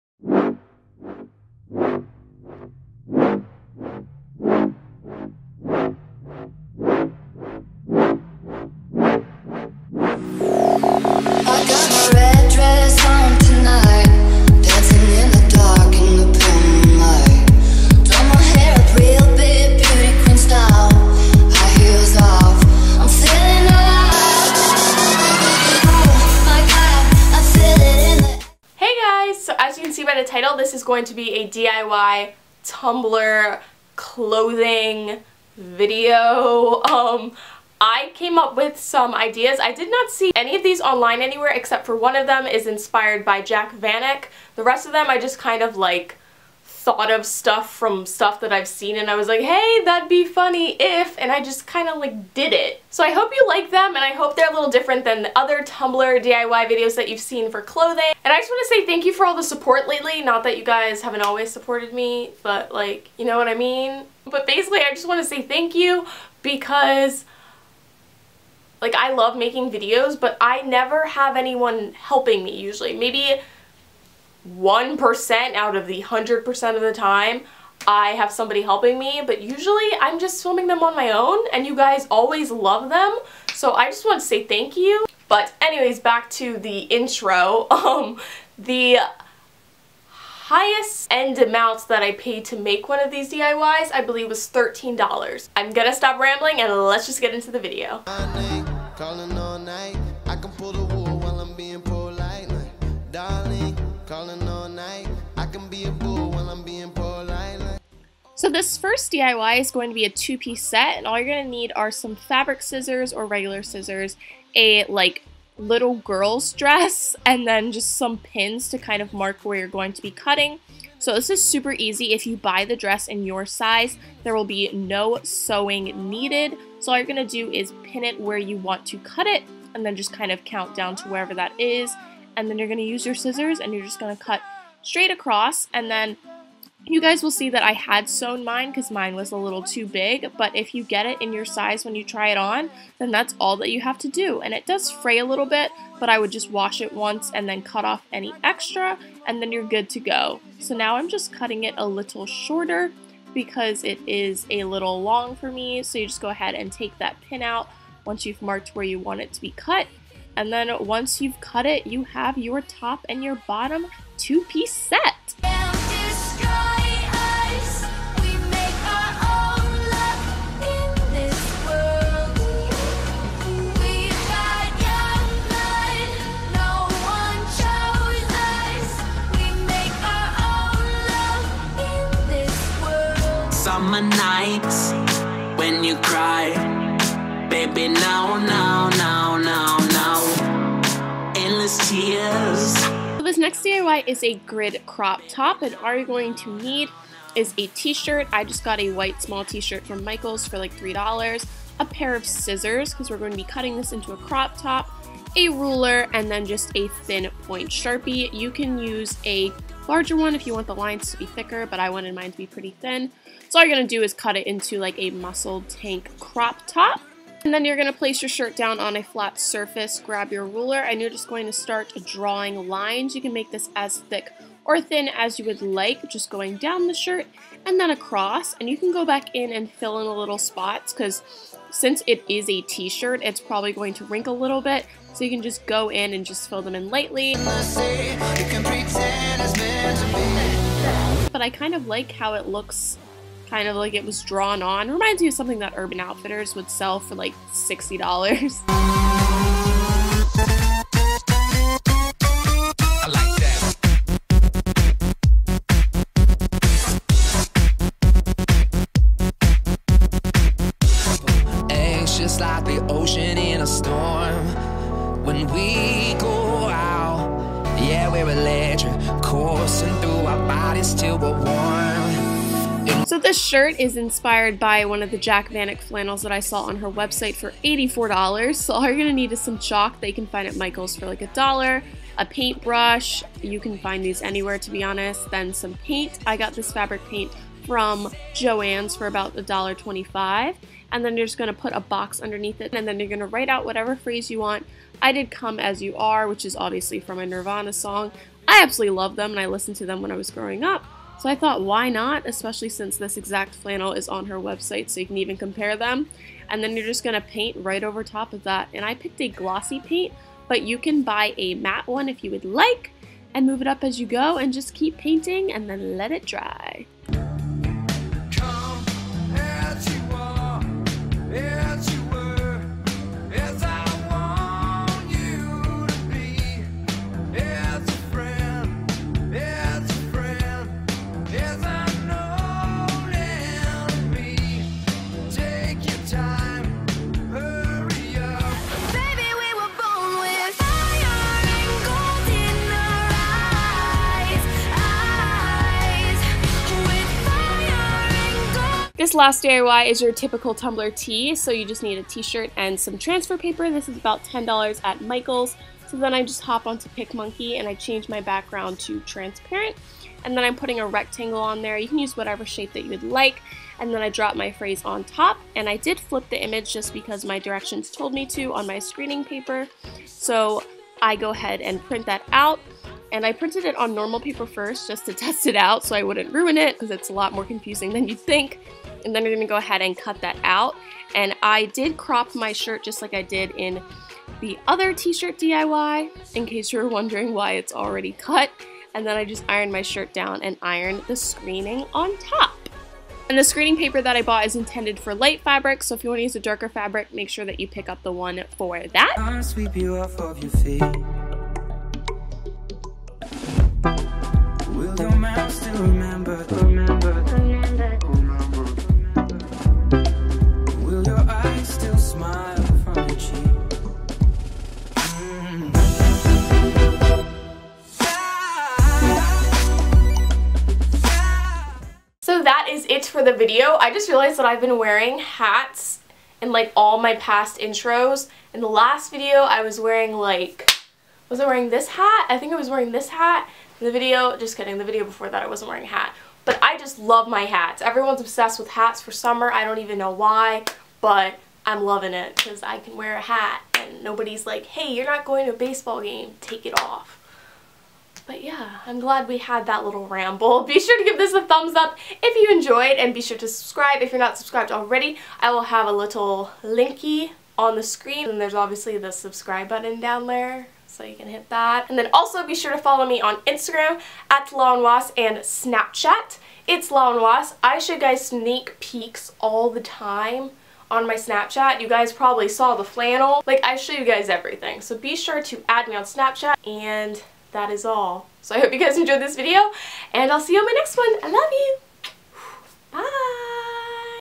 Easy DIY Tumblr Inspired Summer Clothing for Cheap! • Lawenwoss
Hay guys!! Today I am showing you how to create diy "Tumblr Inspired" Clothing for cheap! I show you how to make a matching two piece set, customize a flannel, and create two "tumblr" tees! Let's get this video to 350 likes!

FTC: This is not sponsored and all opinions are my own.